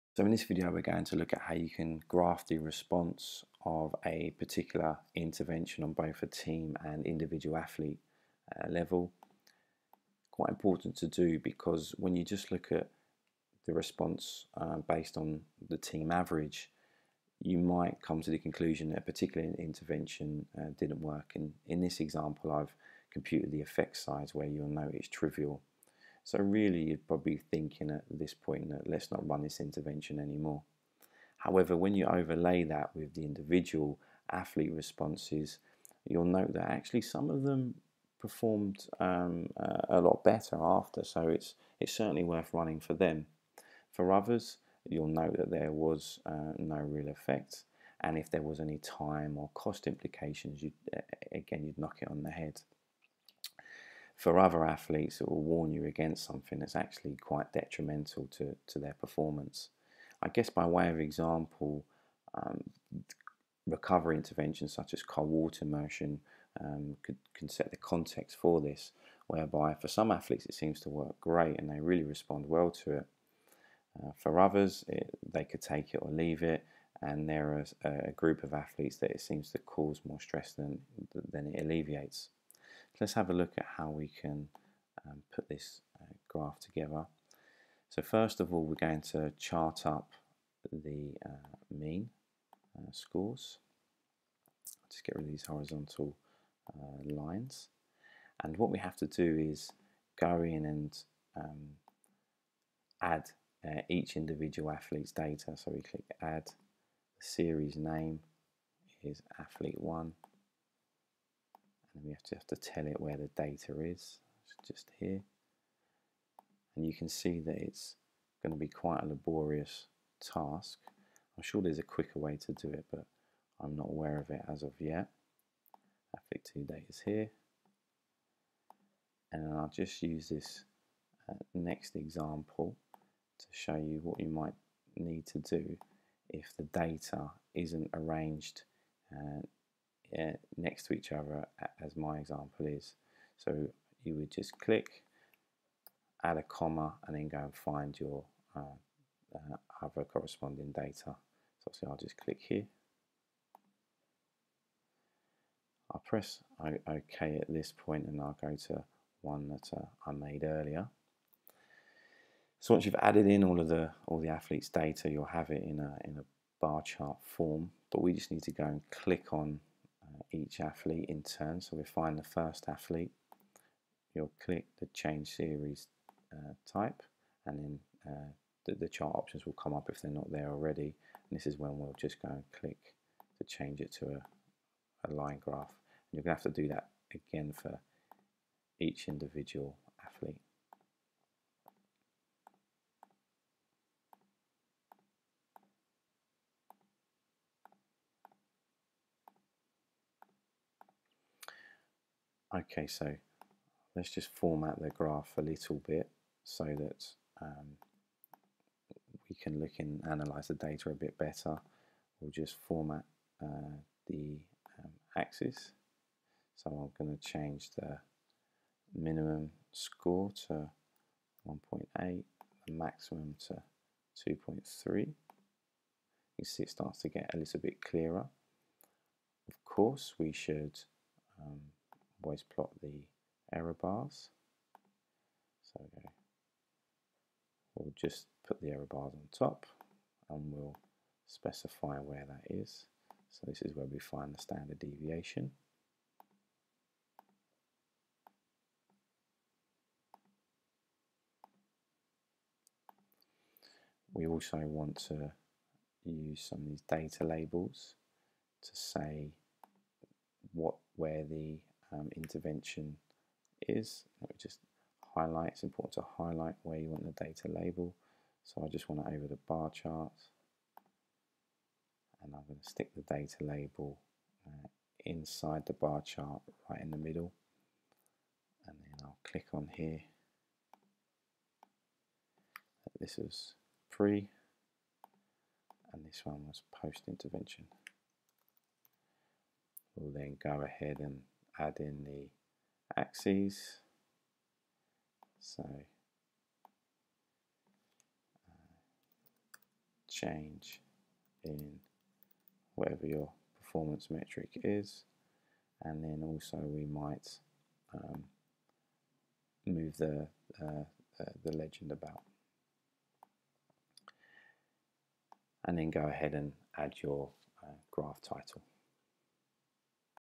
Graphing team vs. individual responses
This video shows how to overlay individual athlete data on top of team average data. It is important to consider how each athlete responds to a particular intervention - average data may be misleading

Just be sure to consider if athletes that show increases or decreases, fall outside the error of the test. We discuss this more in a separate video